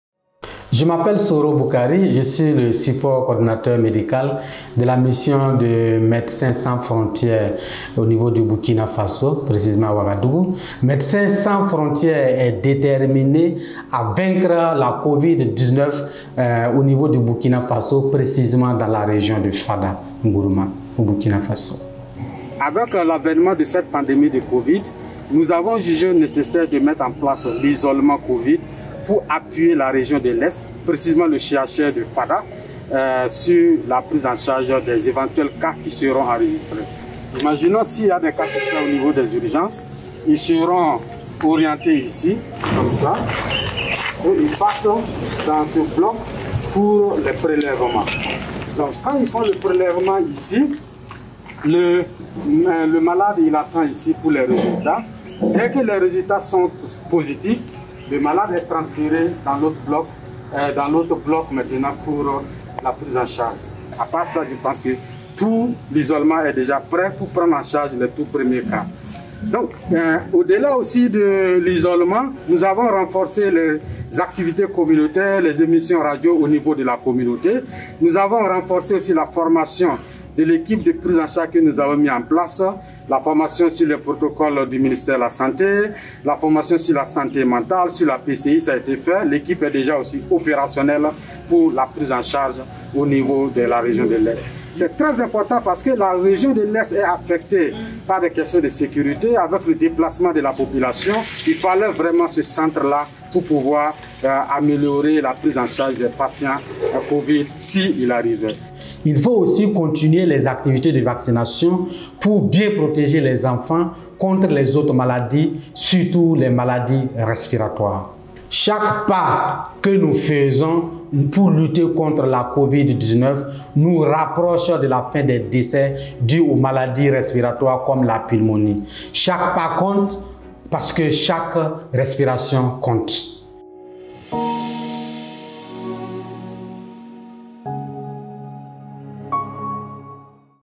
Every Step Counts because Every Breath Counts, Soro Boukary, Burkina Faso
Soro Boukary, Médecins Sans Frontières (MSF), is determined to defeat COVID-19 in eastern Burkina Faso by establishing treatment centers, increasing community awareness, training health workers and continuing with vaccination campaigns.